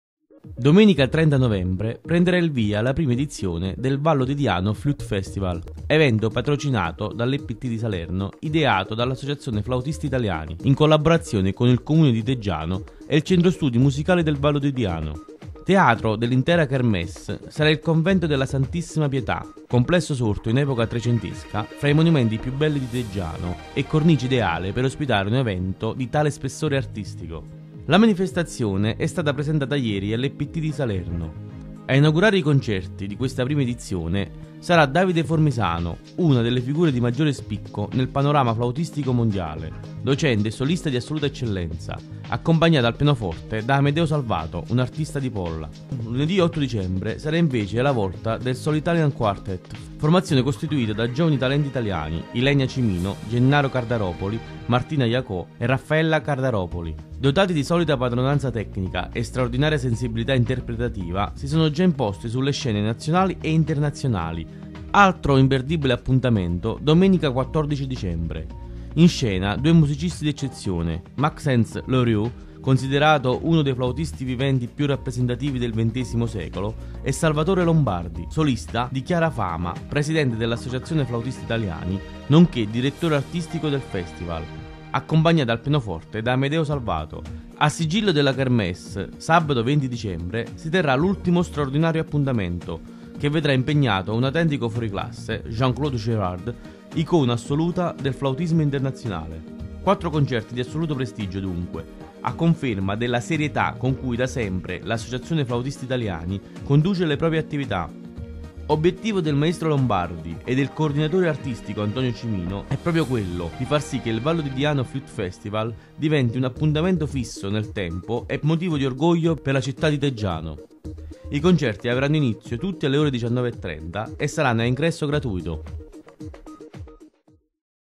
Vallo di Diano Flute Festival. Quattro concerti nello scenario della SS Pietà a Teggiano
27/11/2014